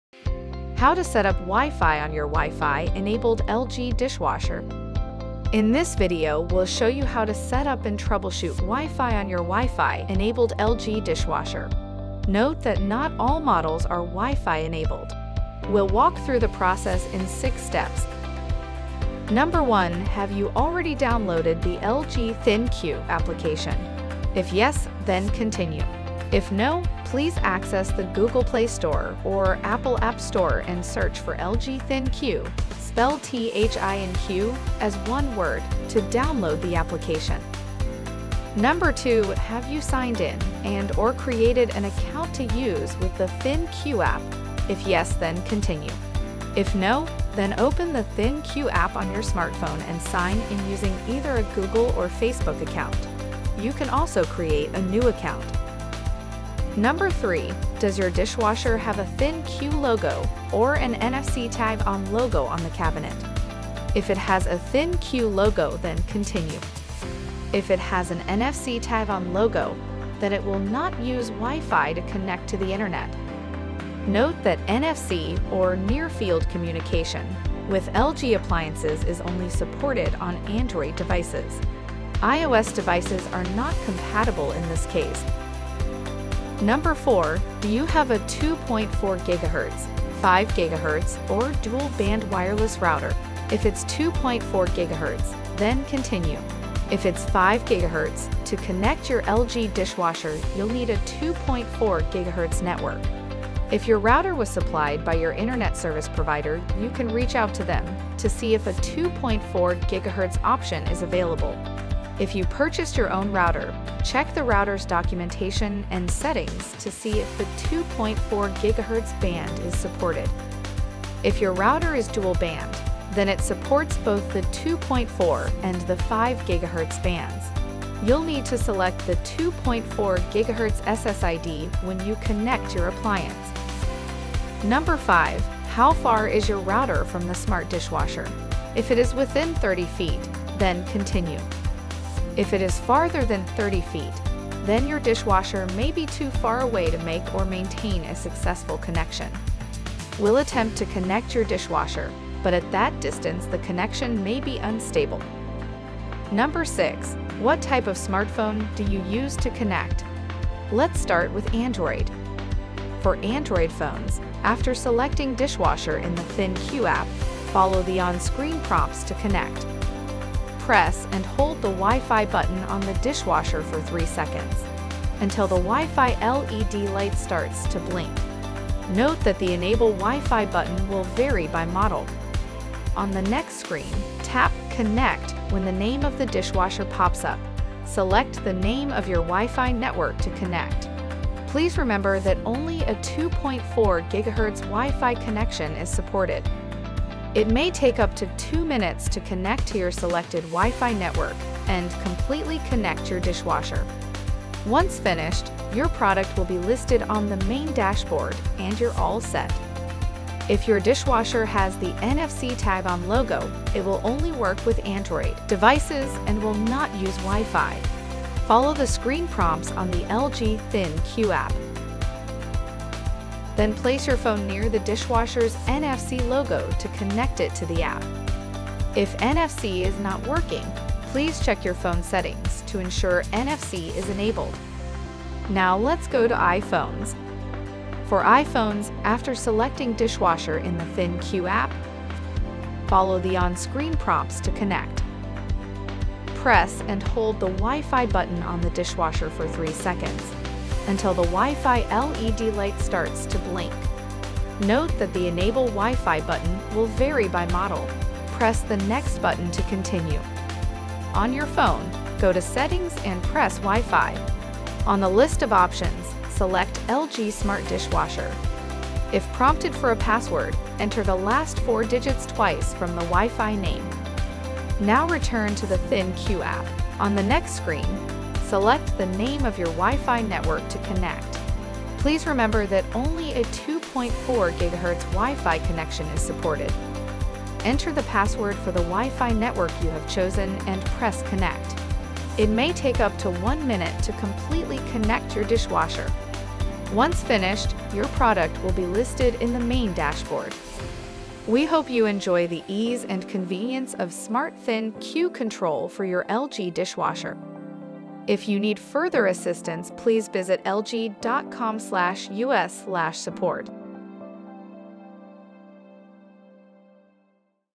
Lavastoviglie LG | Come configurare il Wifi su una lavastoviglie
Video guida per la configurazione e settaggi del wifi nella lavastoviglie LG